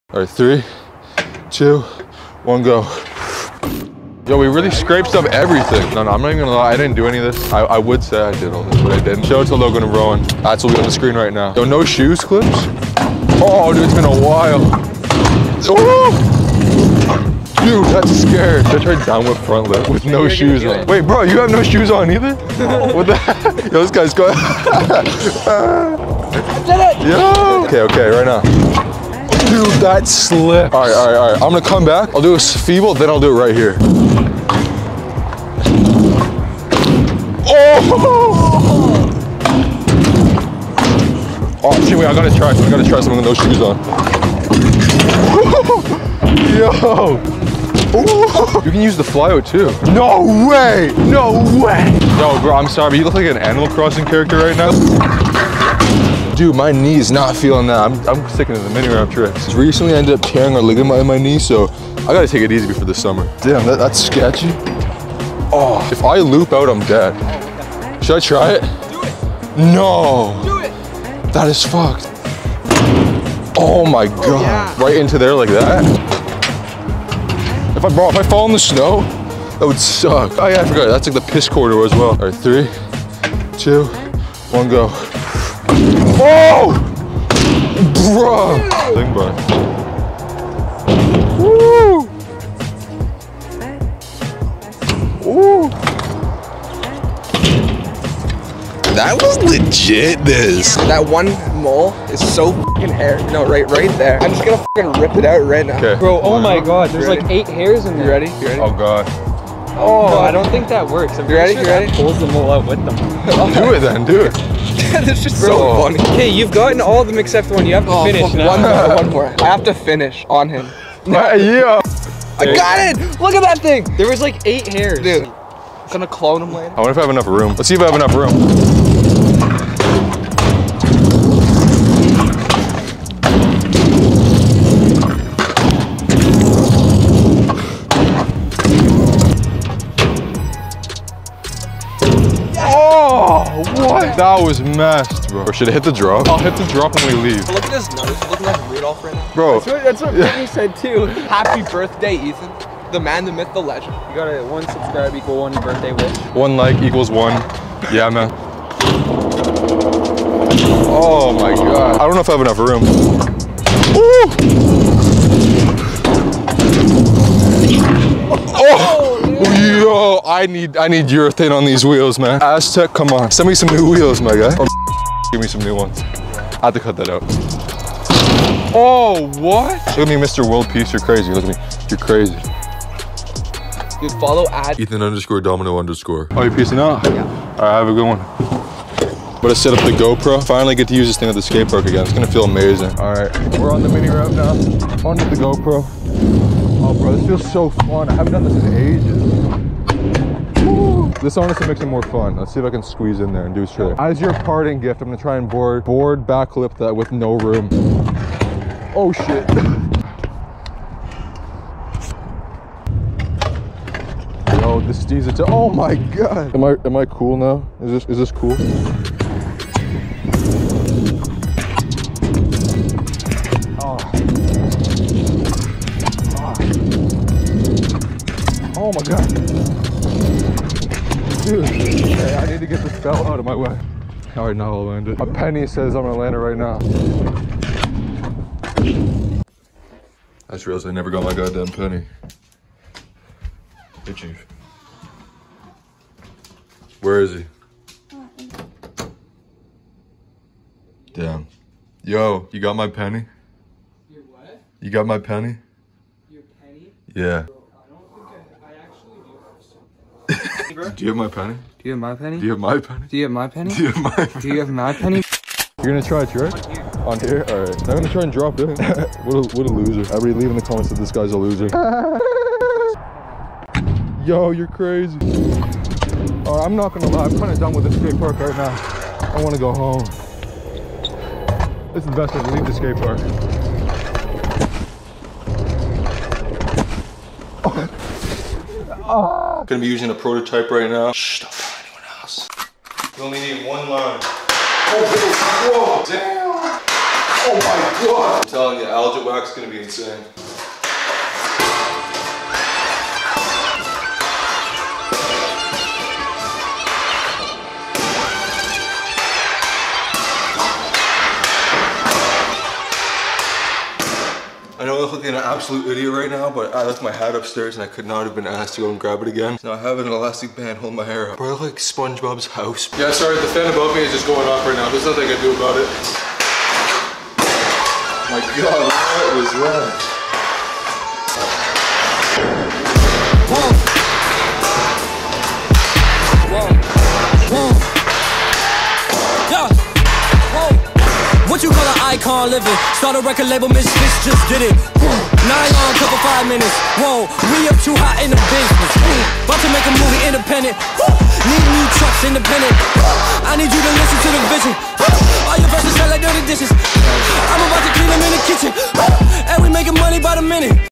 We dug out the skatepark!
Because of all the snow it’s been hard to ride but today we dug out the skatepark.. The mini ramp for more specifics. I had a fun session there with the boys and even got the GoPro strapped on, it felt good to be back at the park filming and shredding! To end it I did a finger whip into the snowbank, I hope you enjoy!